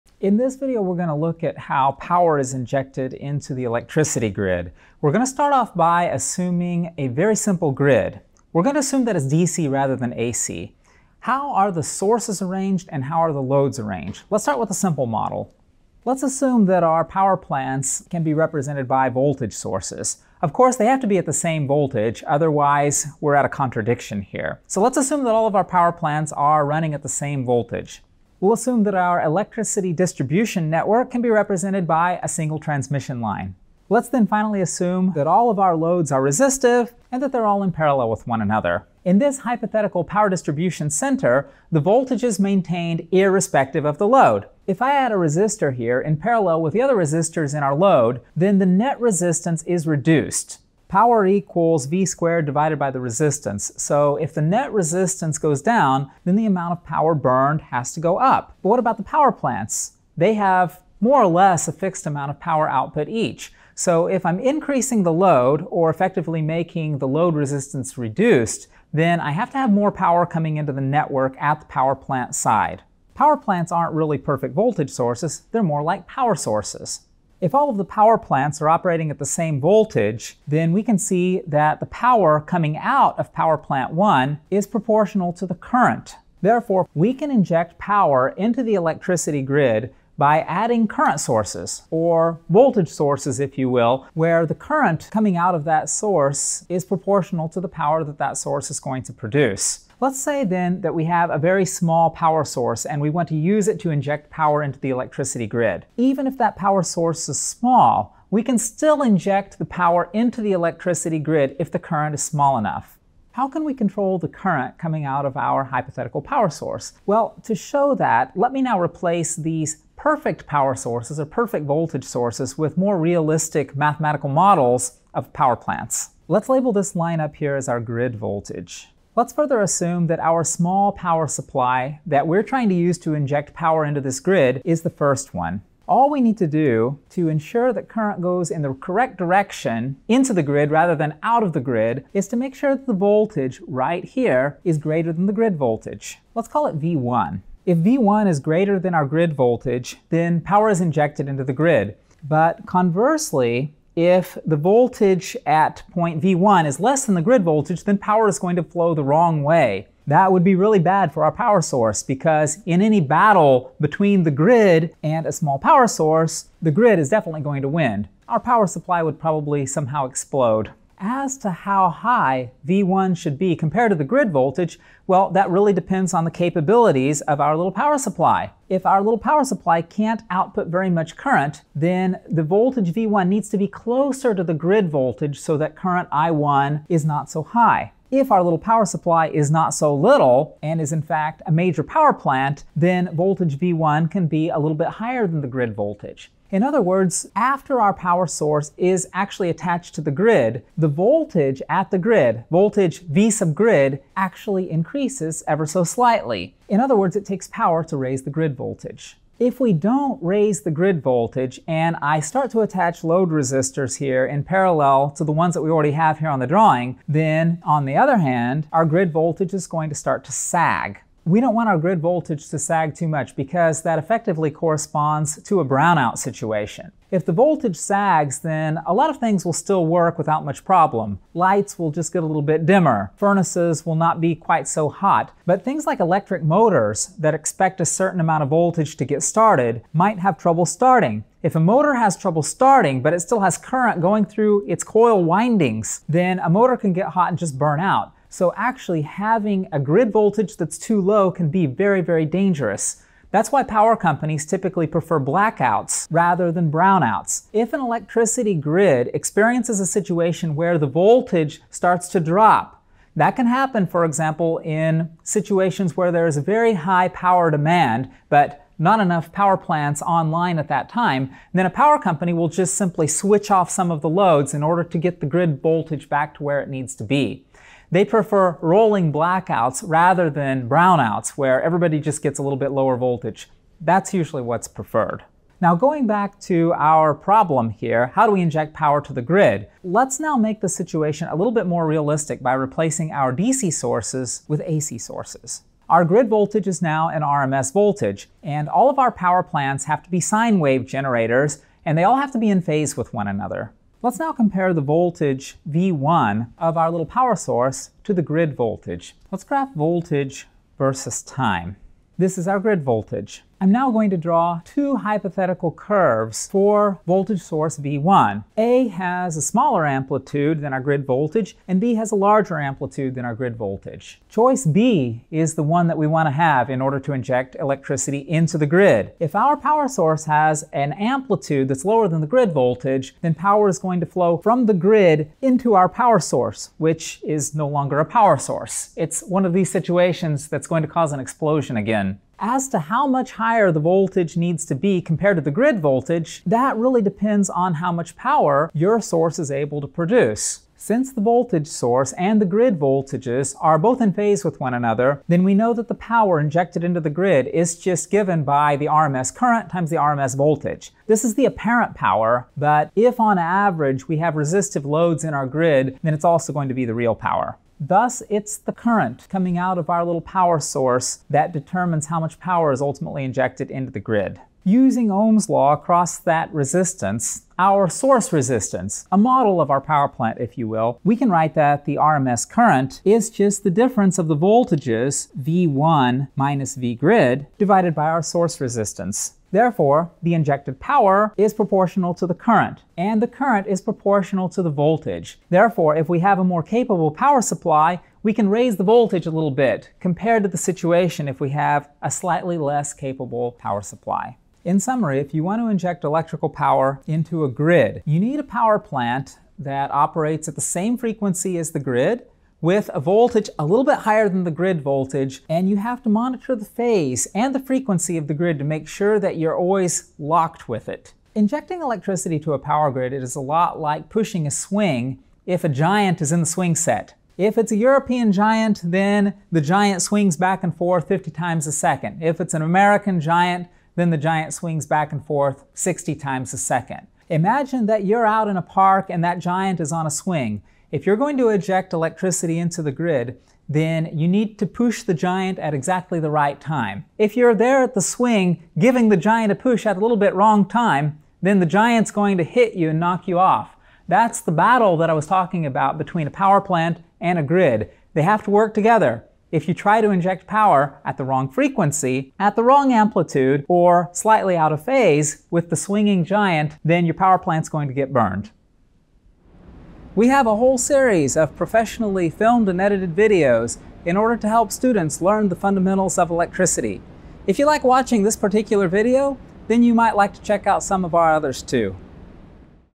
Why electricity flows out of power plants and not back in (27 - Electricity Distribution)
Power flows from power plants to the grid, but what prevents it from flowing the wrong way?  Learn how power is injected into the grid, and why it only flows in one direction.

Video edited by Cheryl Lim.